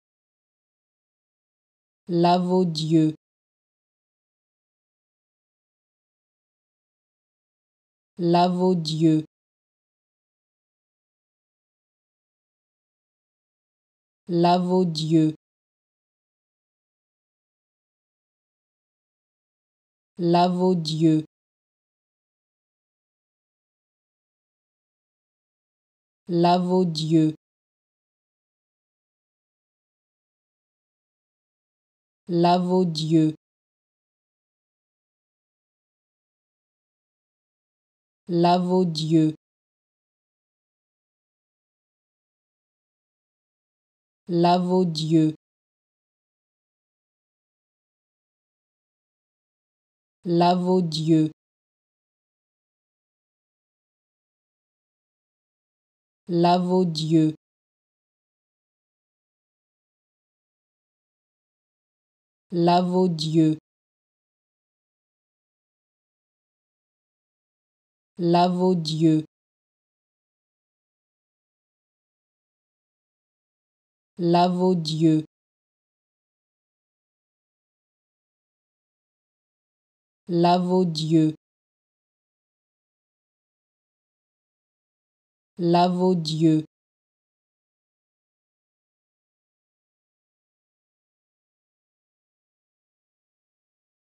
How to Pronounce ''Lavaudieu'' Correctly in French
Learn how to say and properly pronounce ''Lavaudieu'' in French with this free pronunciation tutorial.